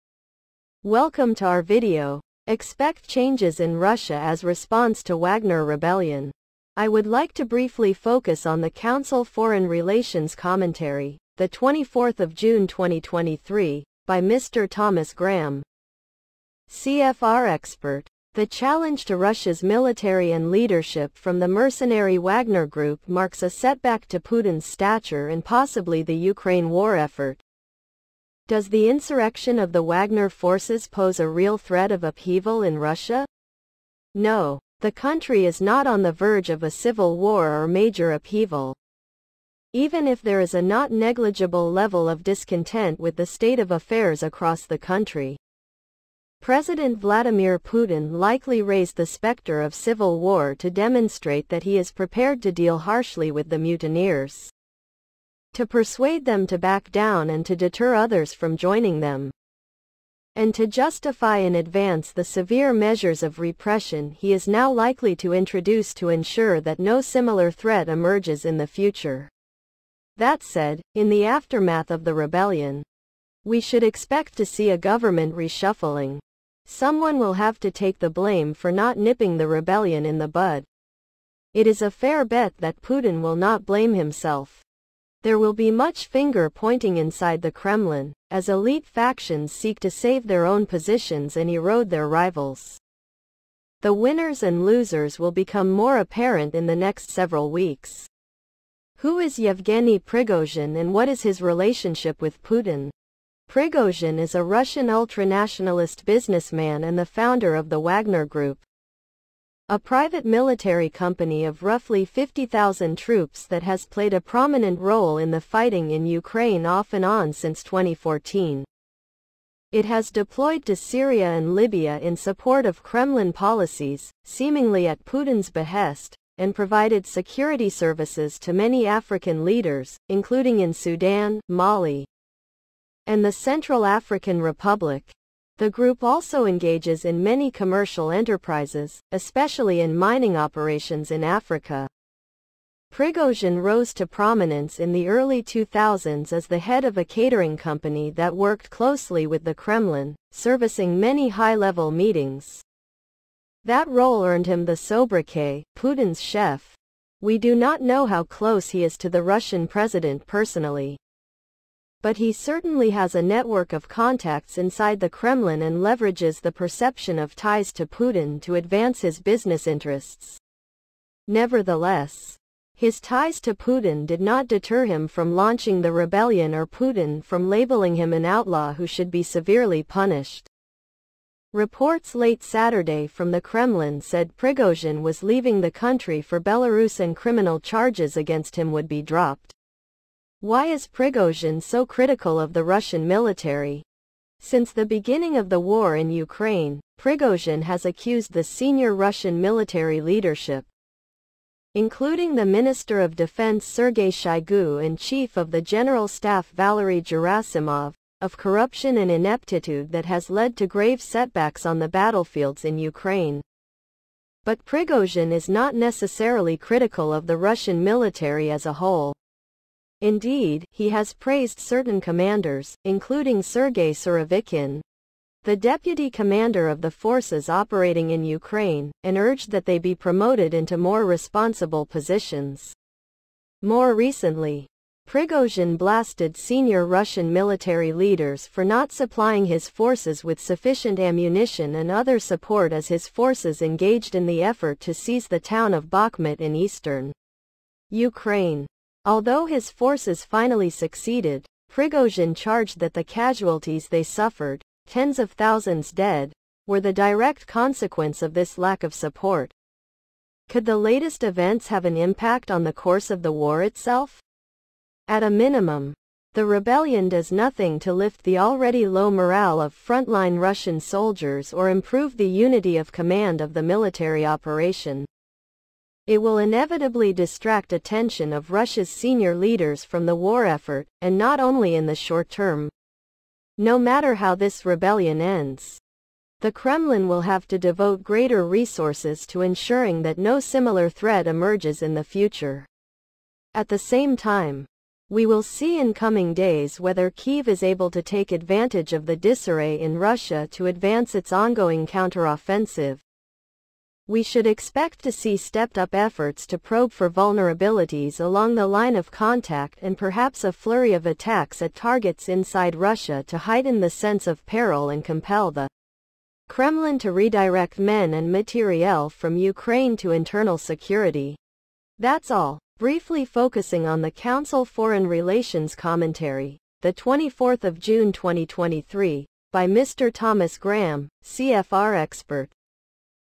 Expect Changes in Russia As Response to Wagner Rebellion.
Council Foreign Relations commentary, 24 June 2023, by Mr. Thomas Graham, CFR Expert.